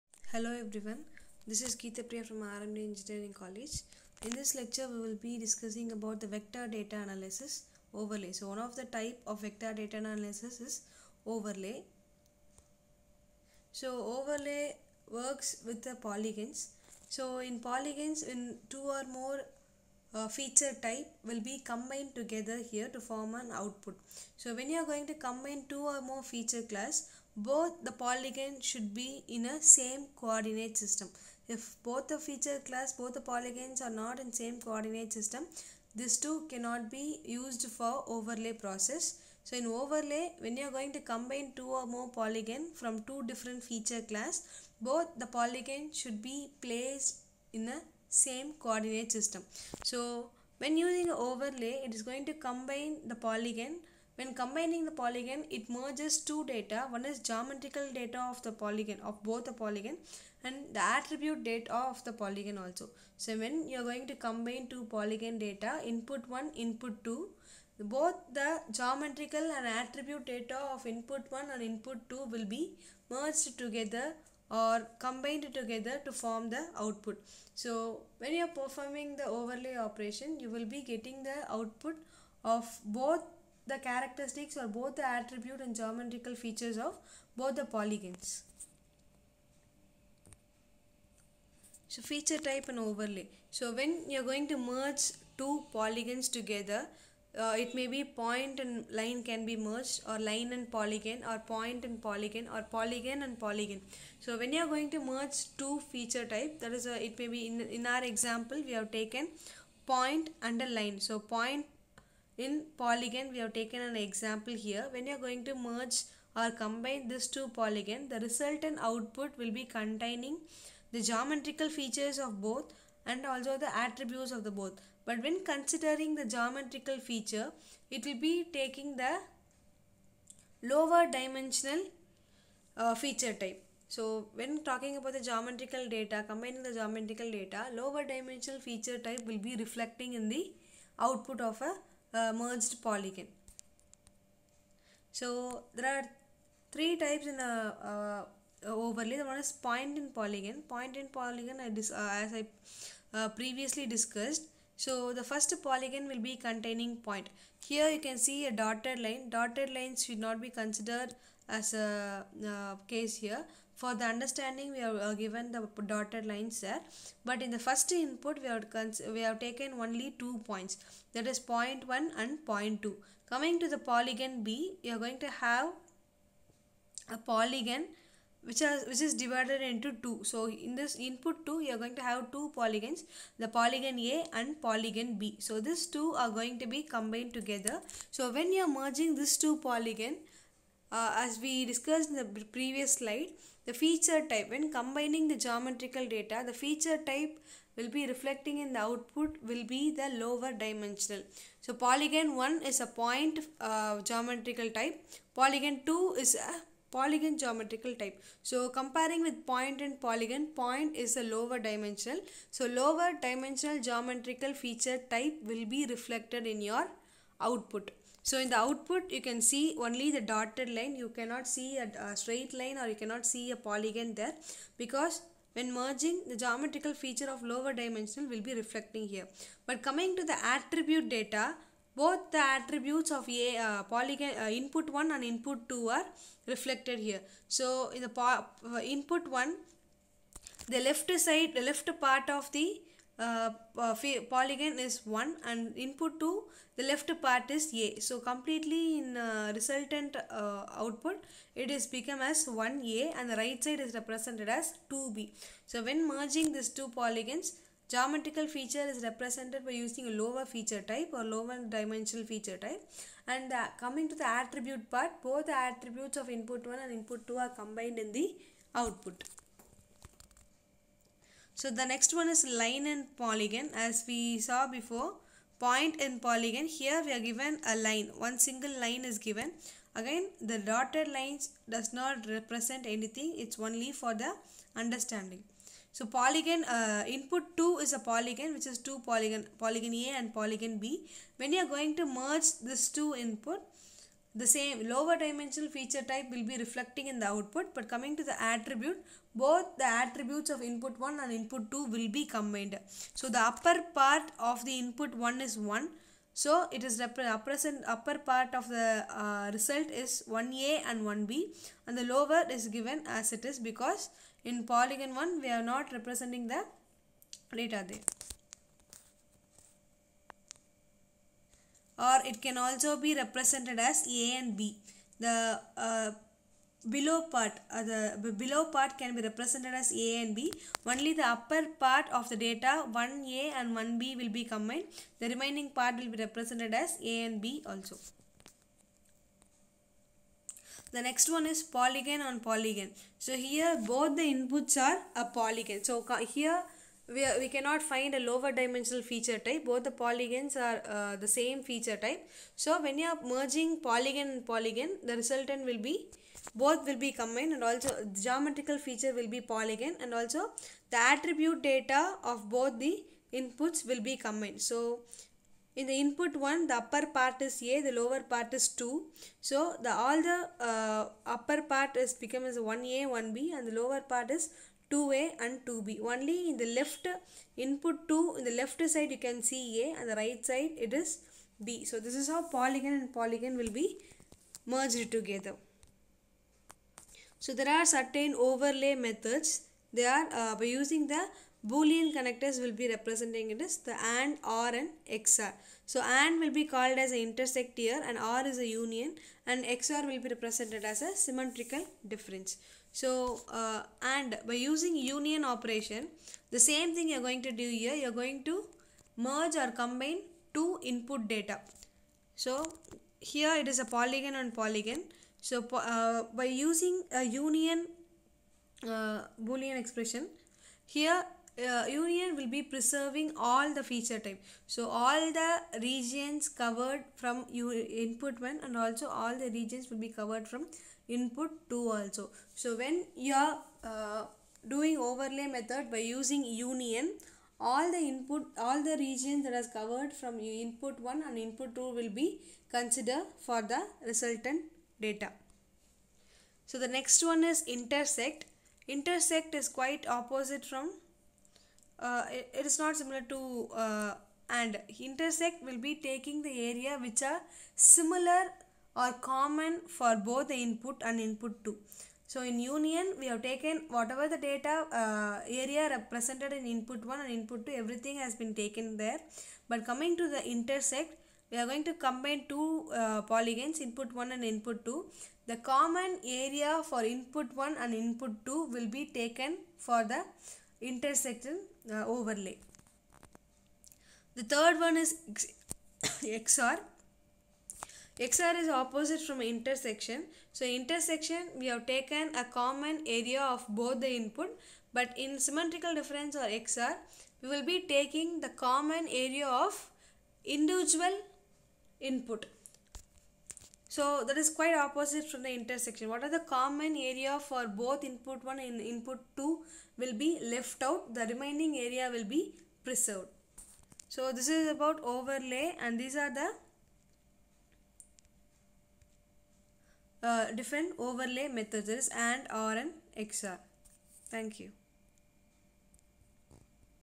Overlay Operation |GIS| Ms. S. Geethapriya, Assistant Professor, CSE, RMDEC
This video explains the concept of Overlay Operation in Geographical information systems